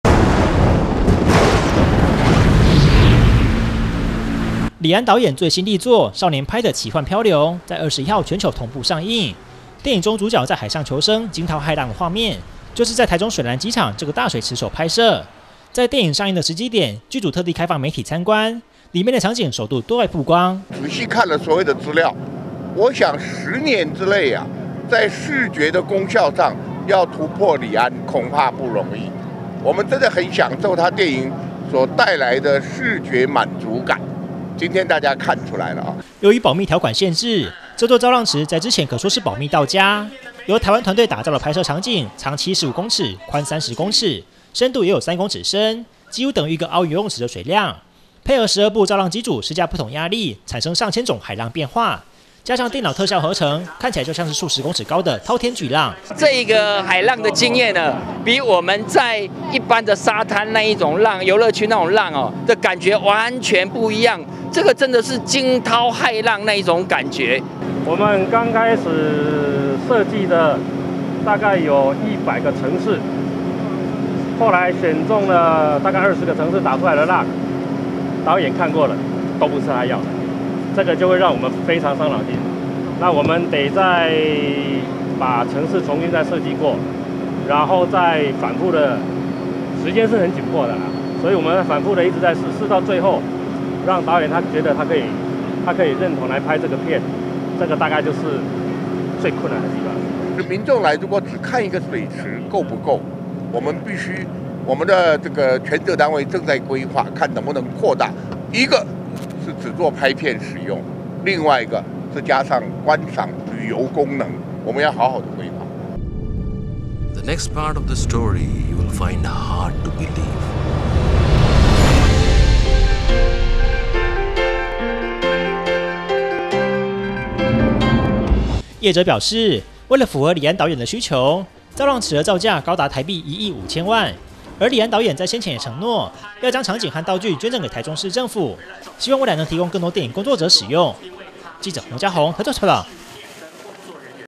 大台中新聞-''少年PI''造浪池全球搶先曝光
金獎導演李安所執導的3D電影，少年拍的奇幻漂流，已經在21號全球同步上映，位在台中水湳機場的拍攝場地"造浪池"，也因為保密條款解除，得以曝光。台中市長胡志強昨天特地帶多位議員前往現場勘查，並且實地上船體驗海浪的威力，整個水池將近7000立方公尺的水量，配合12部造浪機組，交互使用，可以產生近千種的波浪變化，果然讓大家對台灣的工作團隊刮目相看，直呼太神奇了。

以上新聞可由哈TV APP收視